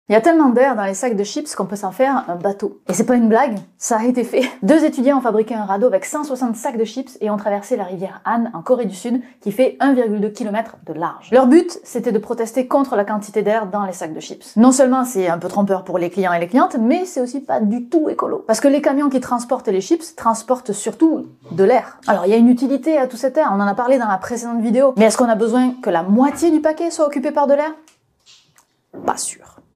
Un radeau en sacs de chips en Corée du Sud
Il y a tellement d'air dans les sacs de chips qu'on peut flotter... la preuve en radeau.